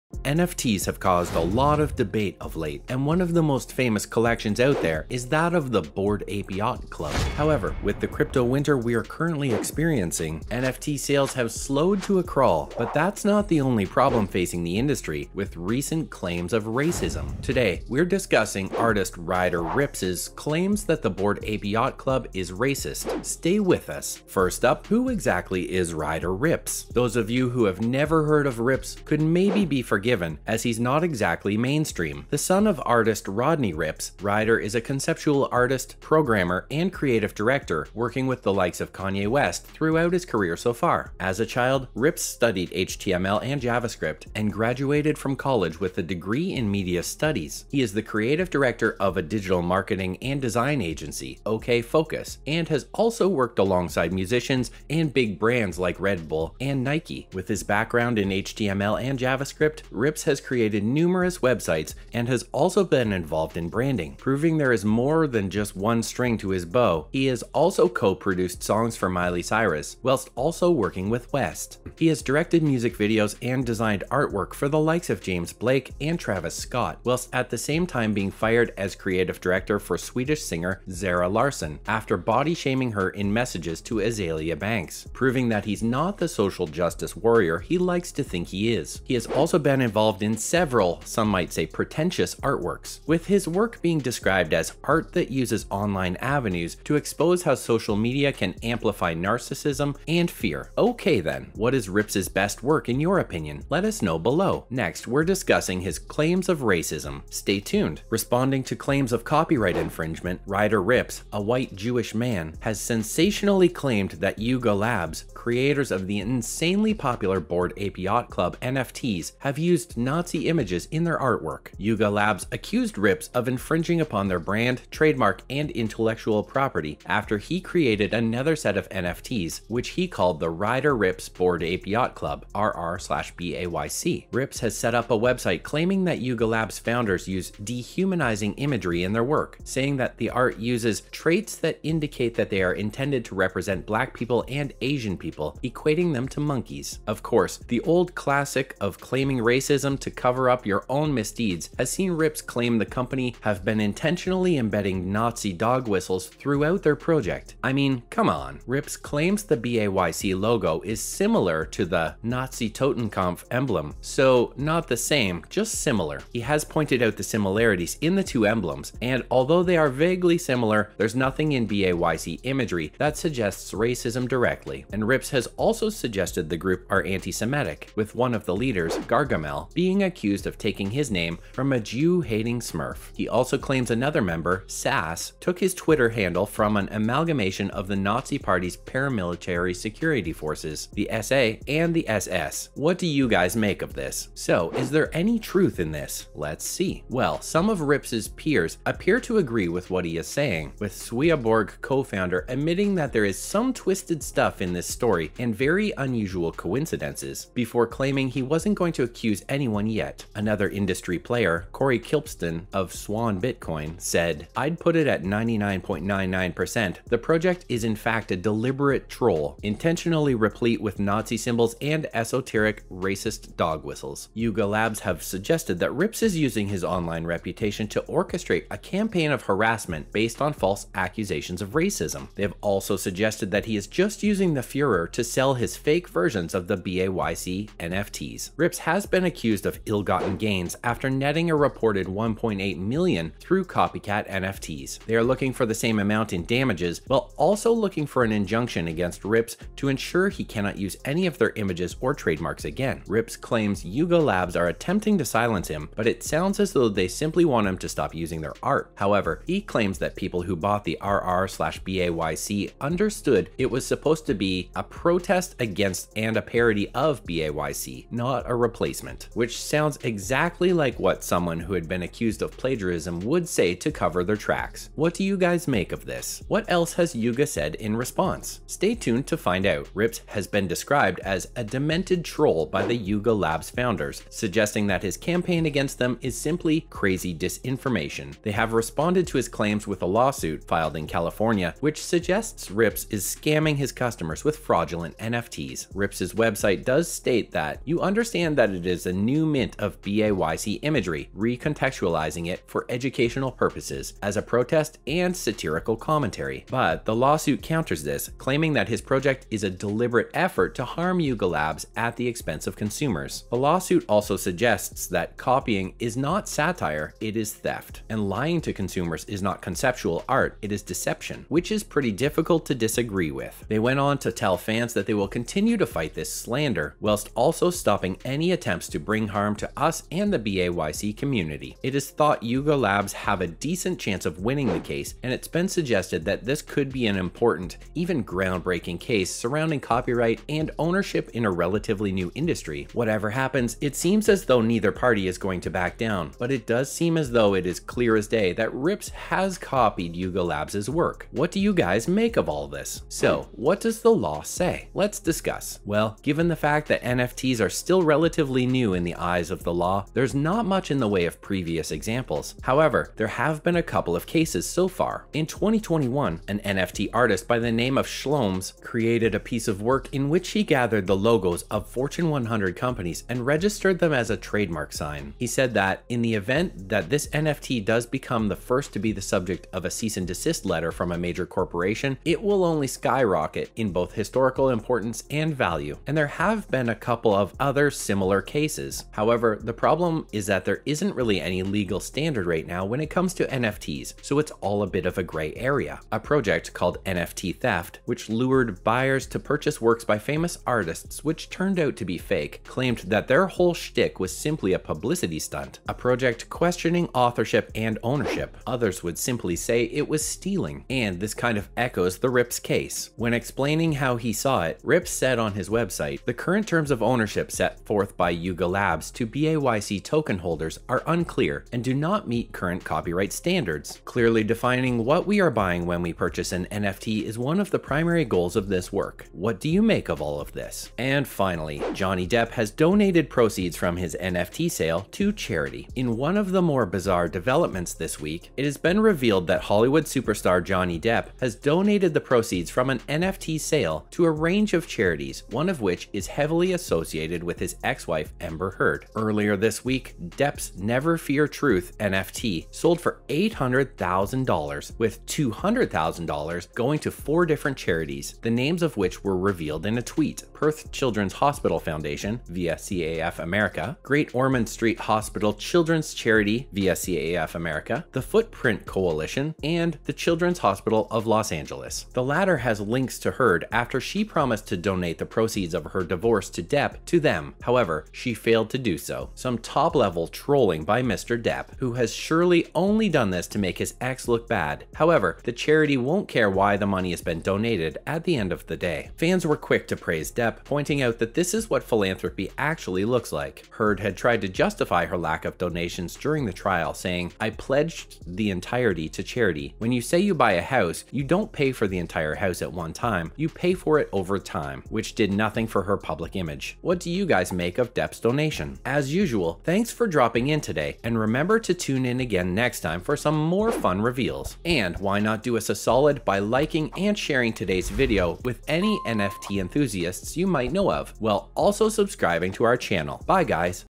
The Bored Ape Yacht Club Has Been Accused Of Being Racist..
Welcome back to The Crypto Merchant, today on the channel we are going to talk about Artist Ryder Ripps Called the Bored Ape Yacht Club NFTs Racist + other related news. In a case that promises to be one of the most hotly watched in the crypto art space, Yuga Labs, the creators of the wildly popular Bored Ape Yacht Club (BAYC) series of NFTs, is suing the artist Ryder Ripps, claiming that he is infringing upon their brand, trademark and intellectual property. Ripps, meanwhile, maintains that the company is attempting to silence critics like him.

The case stems from a copycat collection of NFTs called Ryder Ripps Bored Ape Yacht Club (RR/BAYC), which Yuga Labs says is part of a smear campaign Ripps has been waging online, including what it called false accusations that the origins of BAYC are connected to Nazi imagery.  Ripps is the owner of gordongoner.com, a website named after one of the co-founders of BAYC, on which Ripps posts examples of what he and a handful of others says are the “dehumanizing” elements of BAYC NFTs. Ripps says the works draw on “traits that indicate that they are intended to represent Black people and Asian people… equating them to monkeys,” and that the creators of BAYC have been “intentionally embedding Nazi dog whistles throughout their project.” The information Ripps gathered mostly includes symbols and imagery that he says is gleaned from the history of alt-right/4chan groups involved in crypto. One such image is the BAYC logo, which Ripps says is similar to the Nazi Totenkopf emblem. Watch till the end for more about Artist Ryder Ripps Called the Bored Ape Yacht Club NFTs Racist and other related news.

Bored Ape NFTs are Made by 4chan NEO-NAZIS!? Investigating Why Ape NFTs are using Fascist Imagery